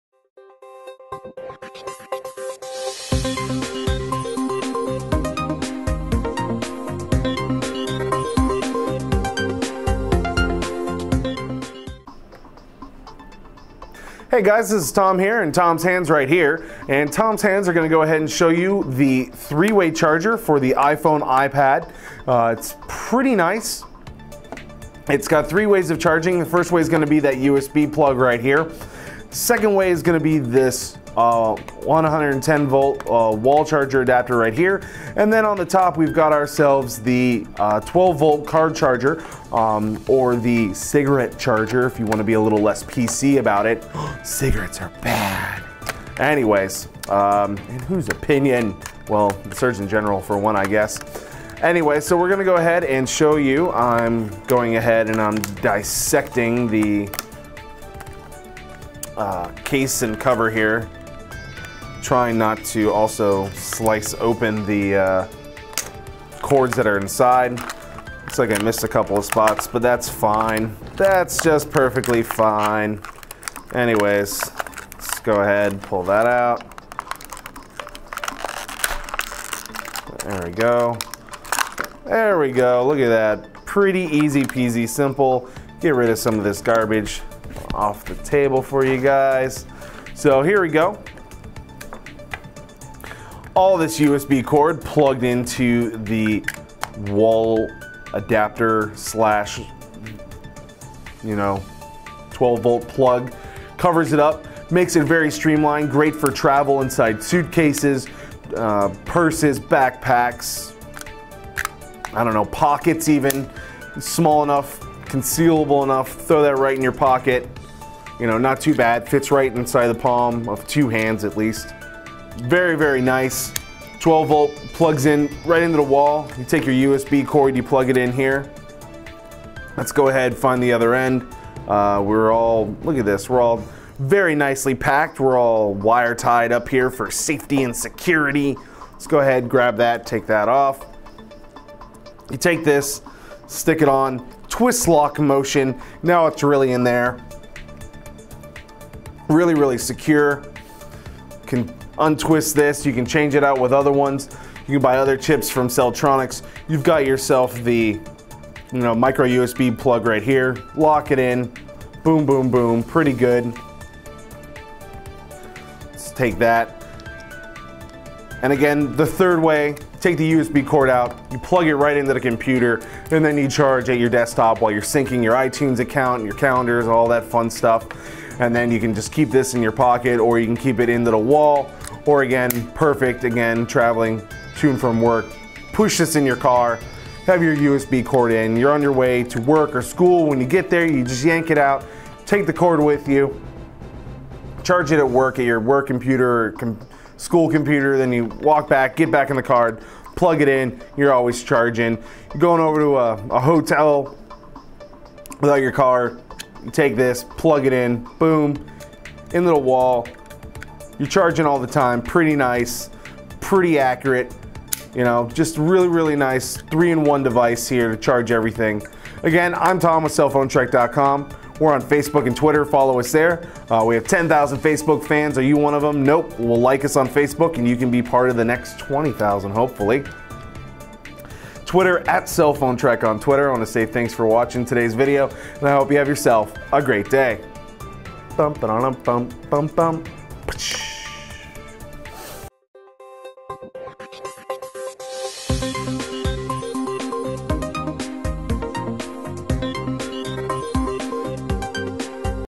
Celltronix Three Way Apple iPhone / iPod Charger
Tommy reviews the Celltronix Three Way Apple iPhone / iPod Charger and shows all the parts and how easy it is you use and carry the charger with you as it is one of the most easy to use and operate chargers around.